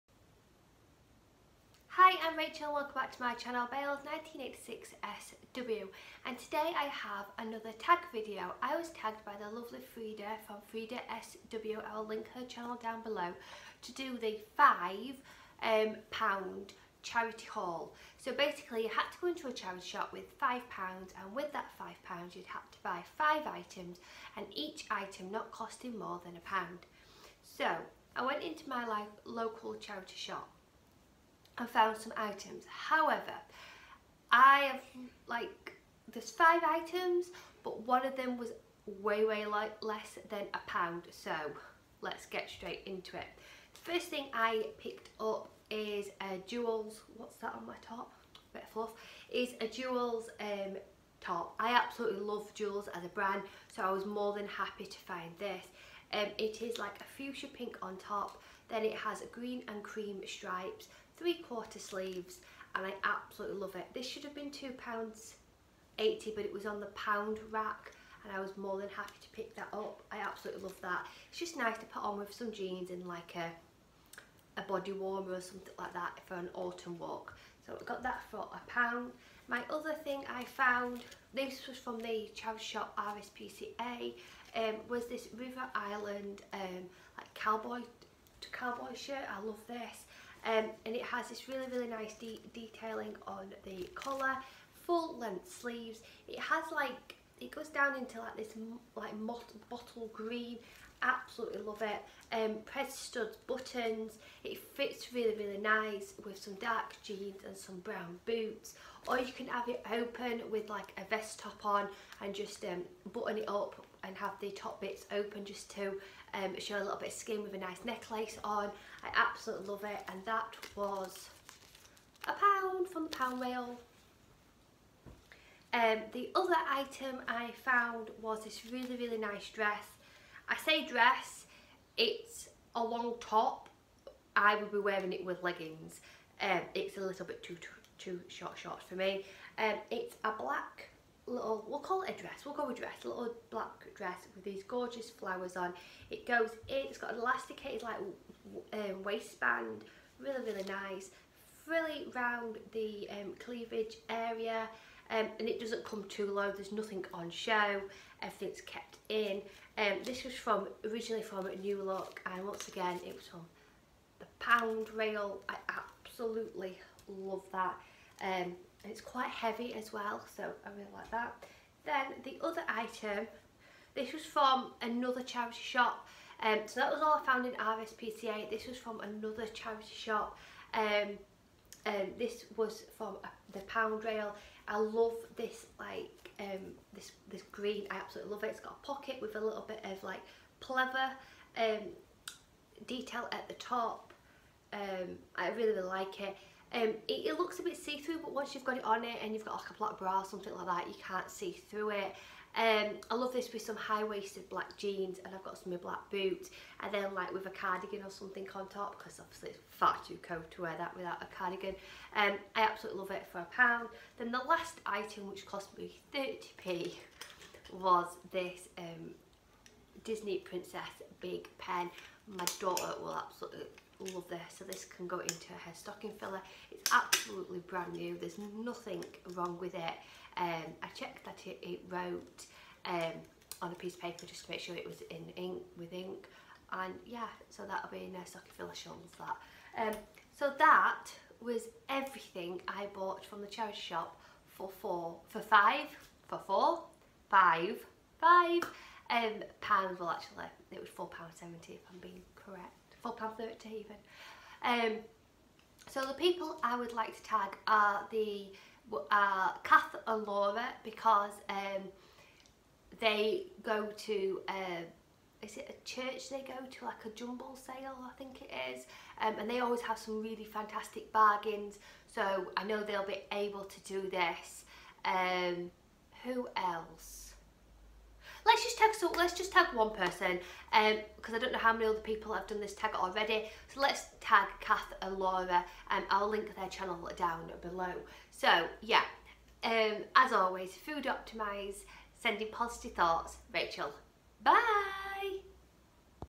Vlogtober Day 23 // £5 Charity Shop Haul Tag // Slimming World
Hello

I was tagged my the lovely lady Freda .

Here is my charity shop tag .

Take care
Sending Positive Thoughts
Rachel